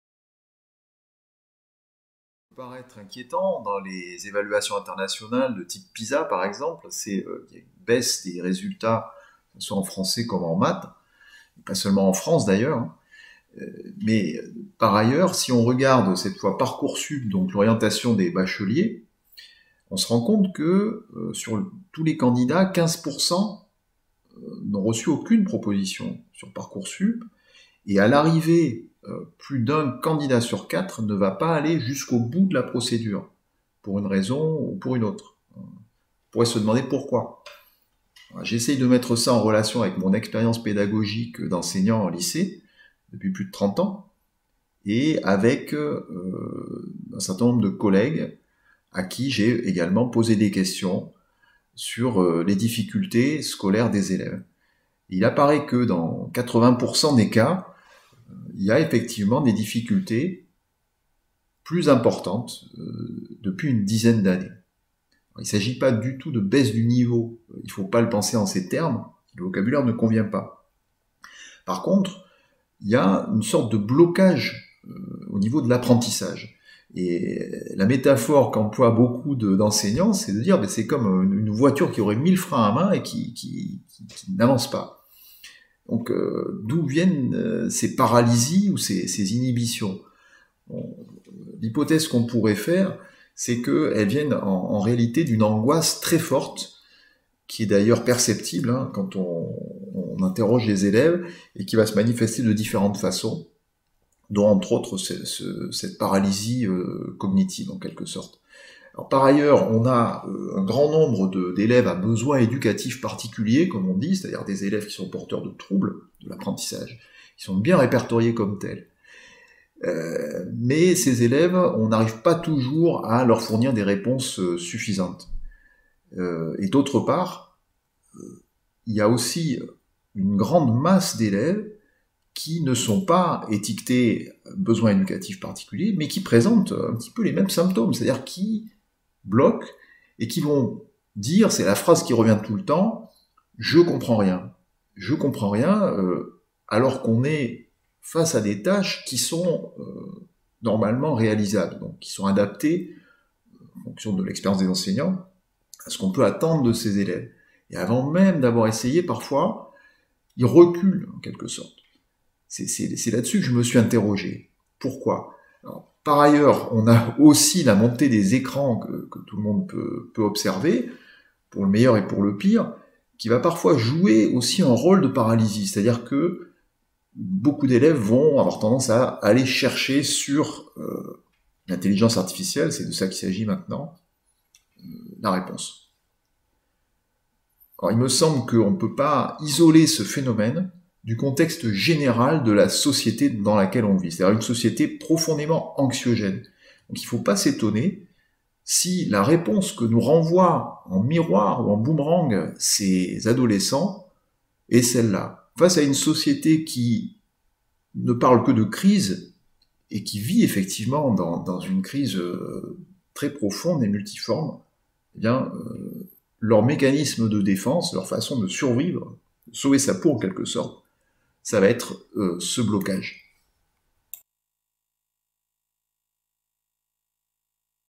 Julien CUEILLE - "Je comprends rien"
Pourquoi les ados résistent-ils aux apprentissages ?

La santé mentale des adolescents est au plus bas aujourd’hui, les angoisses et anxiétés les paralysent et seraient à l’origine de blocage dans leurs apprentissages. Un professeur de philo décrypte les mécanismes de ces résistances.

Beaucoup d’adolescents pourraient se définir ainsi : « Je » suis un être unique, mais à fleur de peau, comme « Mortelle Adèle », je manque de confiance je doute ; je délègue une partie de mes fonctions de pensée, et en tout cas de connaissance, à une intelligence artificielle qui m’accompagne à tout instant de ma vie, toujours et partout.

Comment pourrait-il y avoir un travail sur soi, et même une pensée autonome, si je m’en remets à l’IA et au smartphone pour toute question ?

Souvent, les enseignants parlent des élèves en disant : « c’est comme si on devait pousser une voiture qui a le frein à main ». Les adolescents ont peur d’apprendre.

Derrière des résistances parfois spectaculaires, et des blocages cognitifs généralisés dans certaines classes (« je comprends rien »), se cache une peur panique, celle qui saisit face au radicalement autre, à l’alien. Le savoir scolaire est vu comme un objet étranger. L’identité de certains ados est tellement fragile qu’ils la protègent contre l’intrusion du monde extérieur par un empêchement de penser ; et par des comportements d’auto-mise en échec.

Comment en effet, les jeunes pourraient-ils ignorer la crise dans laquelle ils vivent, celle d’une société « du malaise » où le dérèglement climatique et l’effondrement sont partout ?

Comme le complotisme, qui en dit long, non seulement sur les complotistes, mais aussi sur le système social dont ils sont le produit, la mise en panne, par les élèves, de leurs propres apprentissages reflète également les défaillances du système scolaire, et au-delà.

Dans la collection

Enfances, parentalités et institutions